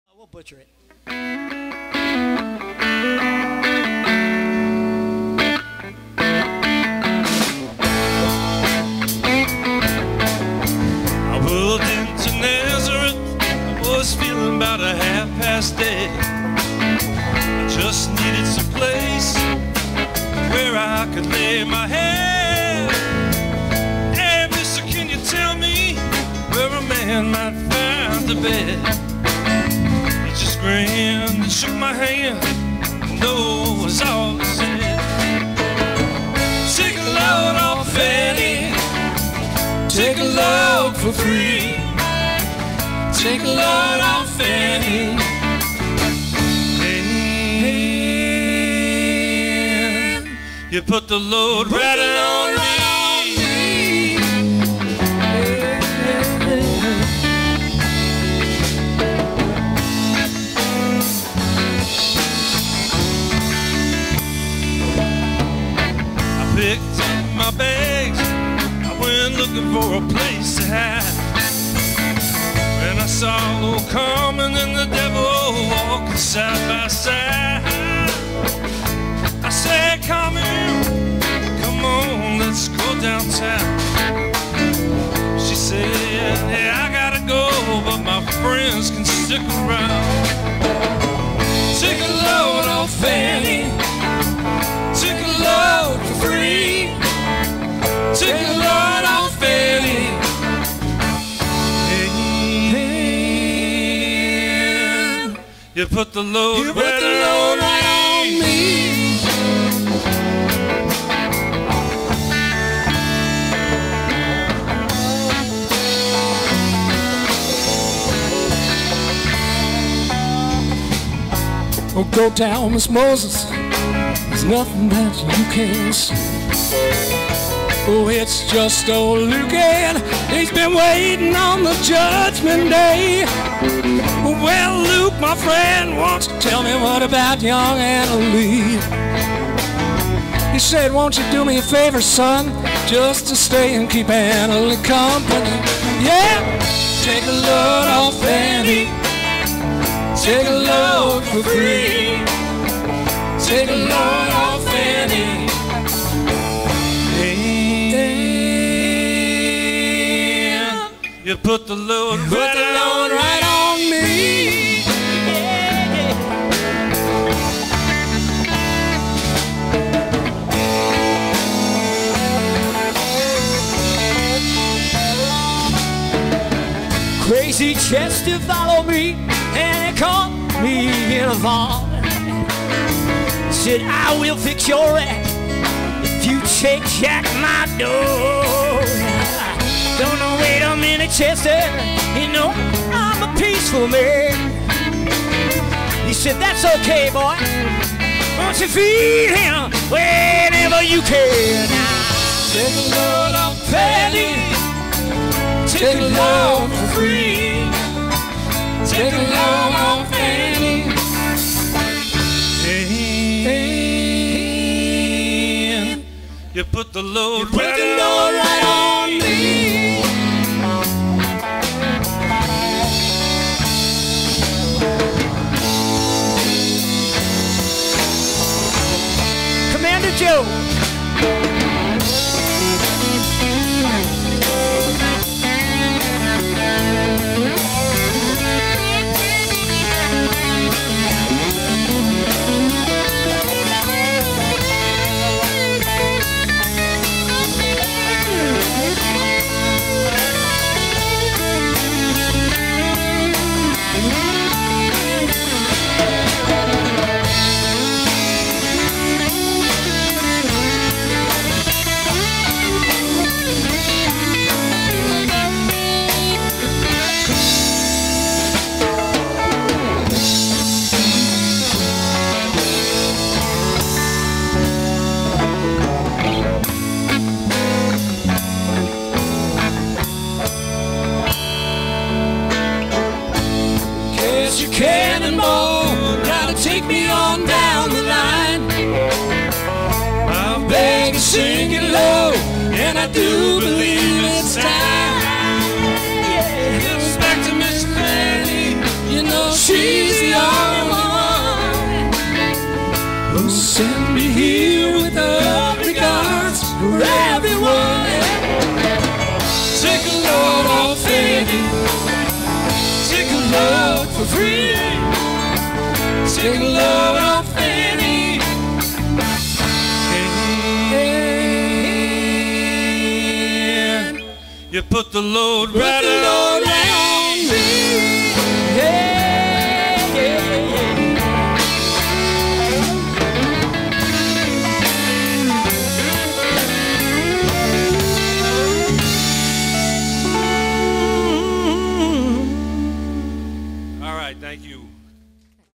The Weight Summer Jam 2017
The first Summer Jam at the Vengen/Campion/Moore compound in Oak Ridge NJ.  Playing tribute to Greg Allman, brother Duane Allman, Sandy Denny, Chuck Berry, John Denver, Jerry Garcia and many others.